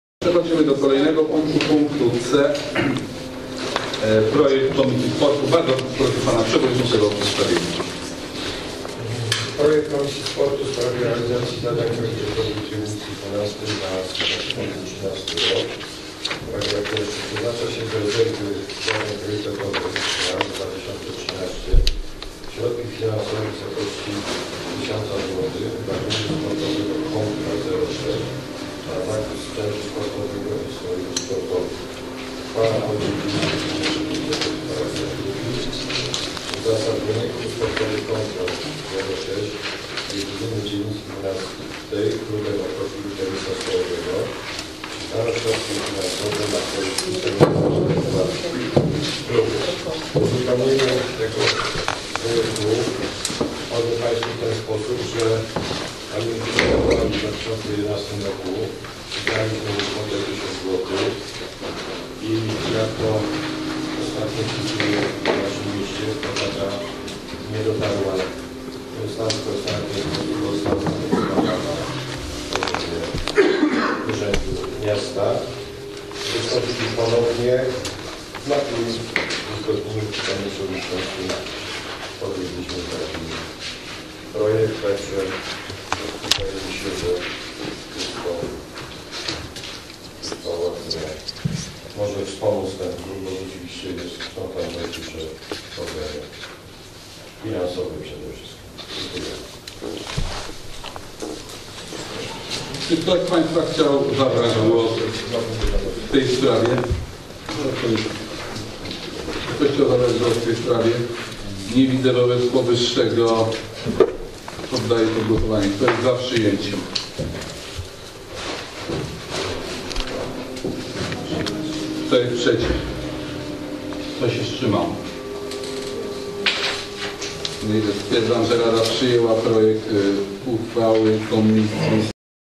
RD_XII_ 15-012-013_5 Wsparcie finansowe dla Klubu sportowego CONTRA 06
Obrady XXX sesji Rady Dzielnicy XII Bieżanów-Prokocim z dn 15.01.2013 r.
część 5:
Uchwała w sprawie Wsparcia finansowego dla Klubu sportowego CONTRA 06